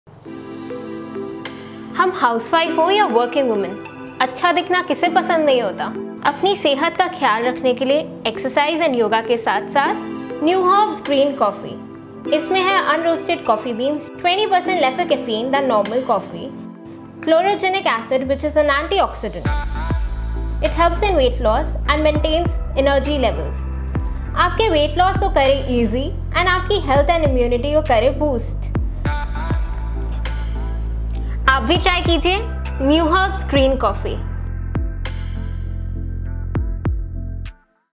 Maintain Your weight with neuherbs Instant Green Coffee Premix
Now losing those extra kilos is an easy task with neuherbs Instant Green Coffee Premix. It is plant-based and uses raw and unroasted green coffee beans with associated health benefits.
Essential Benefits-
- Green coffee is known to possess chlorogenic acid (an antioxidant) which helps boost metabolism & hence assists in weight management.
- Help improve insulin function, which assists in maintaining blood sugar levels post high carb meal.
- The richness of antioxidants flushes out toxins & makes it perfect for body detoxification.
Take 1 cup of green coffee before your heavy meals or post-workout.